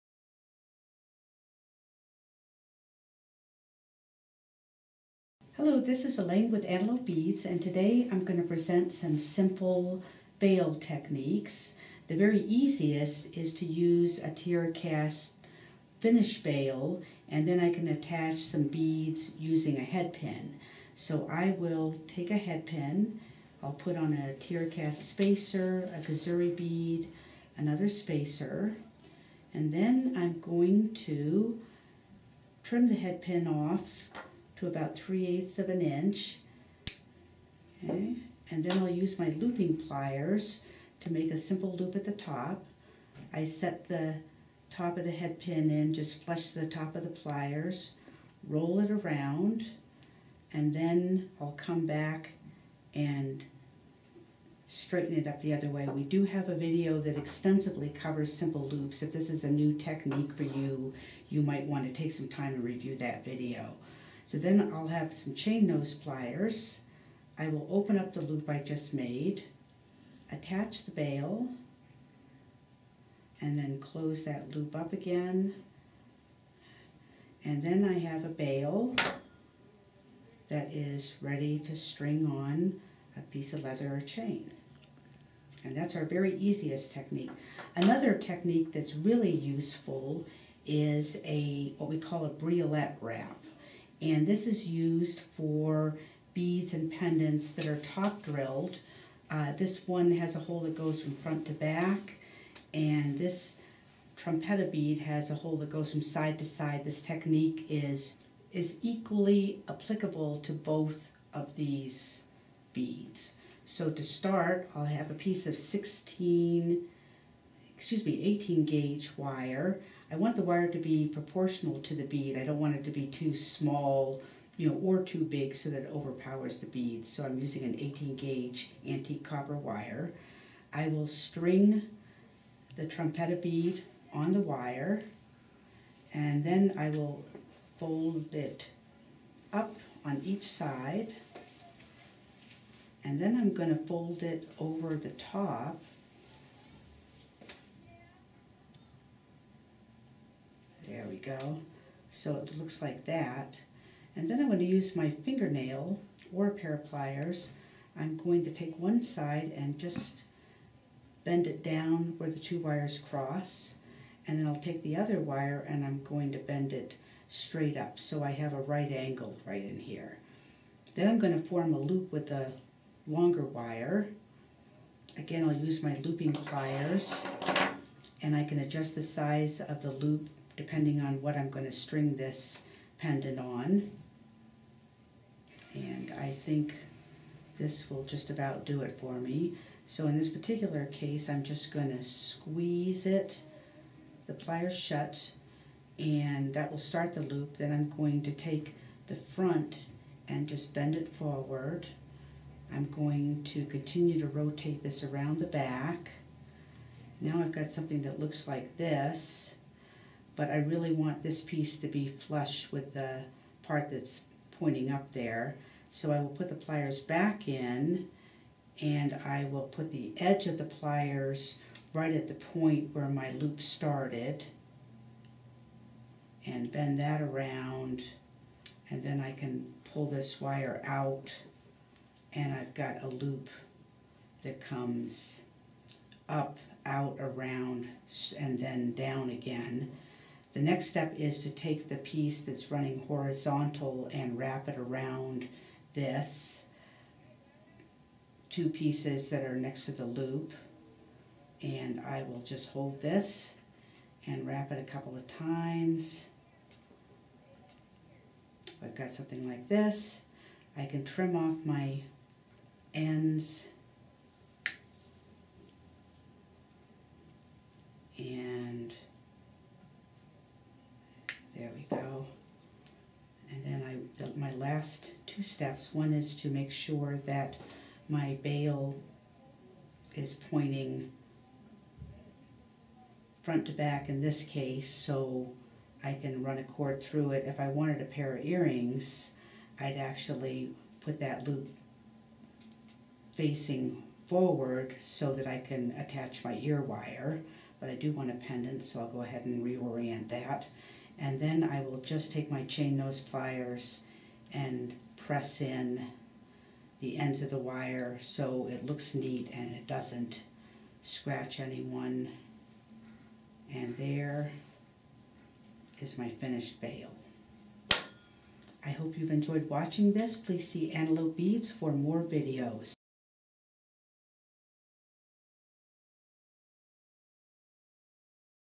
Antelope Beads - How To Make A Briolette Bail For Jewelry Making
AntelopeBeads.com - Custom Bails are an essential jewelry making technique that are easy to learn. In this video Elaine shows you how to make a simple Briolette Bail.  Happy Beading!